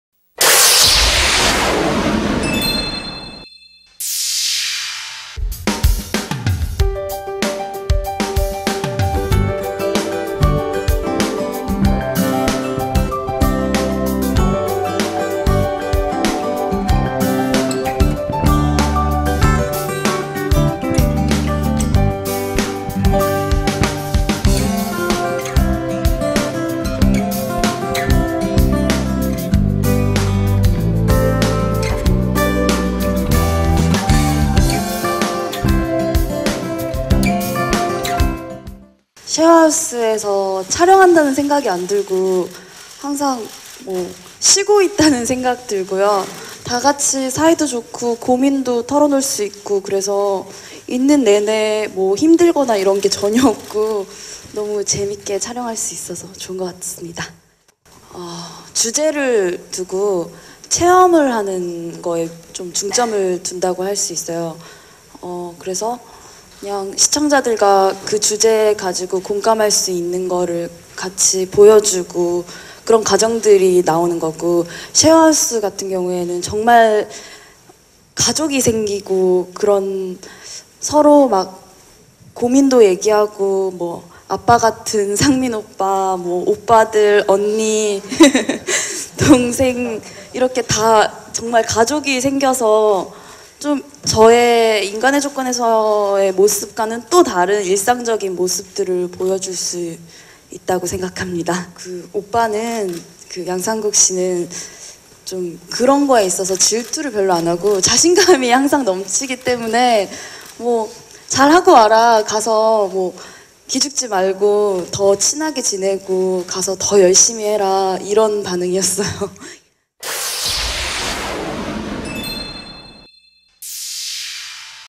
[tvdaily] '셰어하우스' ★천이슬★ "양상국, 질투? 자신감 넘쳐 오히려 응원"
[티브이데일리 오수정 기자] '셰어하우스' 천이슬이 남자친구 양상국을 언급했다.

14일 오후 상암동 CJ E&M 센터에서 케이블TV 올리브 '셰어하우스' 제작발표회가 진행됐다. 이날 행사에는 이상민 최성준 최희 천이슬 우희(달샤벳)와 이수호PD가 참석했다.

이날 천이슬은 "최성준 이상민 손호영 등 멋있는 분들이 많은데 '셰어하우스'에 입주할 때 양상국의 반응이 어땠나"라는 질문에 "다른 방송에서도 말한 적이 있는데 양상국은 자신감이 넘쳐서 질투를 거의 안 한다"며 "이번에도 '가서 더 열심히 하고 기죽지 말라'고 말해줬다"고 말했다.

'셰어하우스'는 각기 다른 삶을 살아온 셀러브리티들이 한 집에 모여 살며 펼쳐지는 일상을 담은 리얼리티 프로그램으로 현대사회에서 희미해진 '식구'란 재조명하고자 한다.

'셰어하우스'에는 방송인 이상민, 가수 손호영 우희, 전 아나운서 최희, 배우 최성준 천이슬, 모델 송해나, 디자이너 황영롱 김재웅이 한 공간에 모여 두 번째 식구를 만들어가는 과정을 그려나갈 예정이다. 오는 16일 밤 9시 첫 방송.